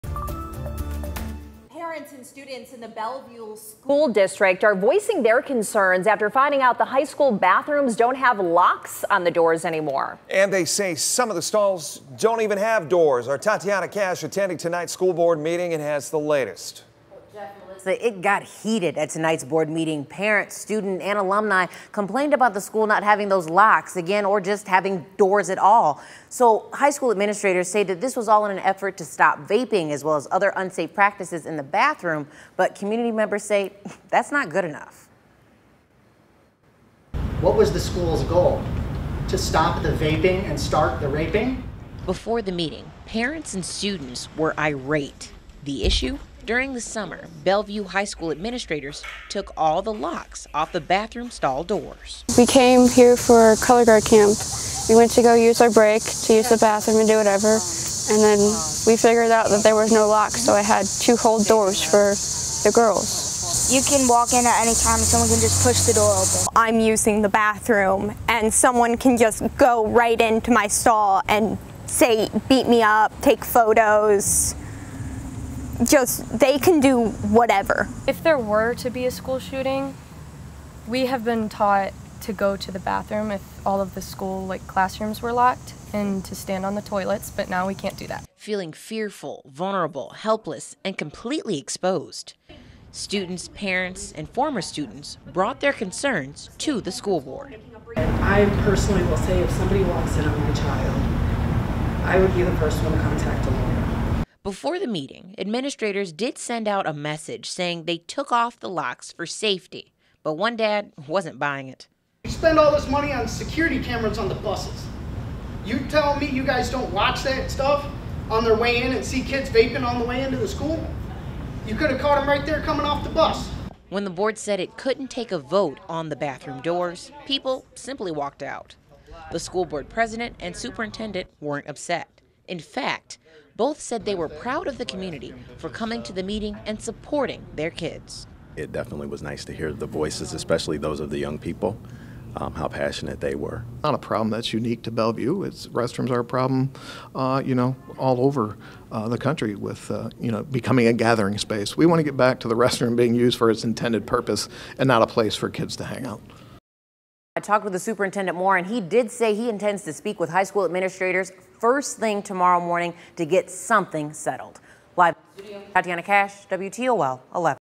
Community outrage over Bellevue High School bathrooms with no locking doors | WTOL 11
Parents and students in the Bellevue School District are voicing their concerns after learning the high school bathrooms don't have locks on the doors anymore.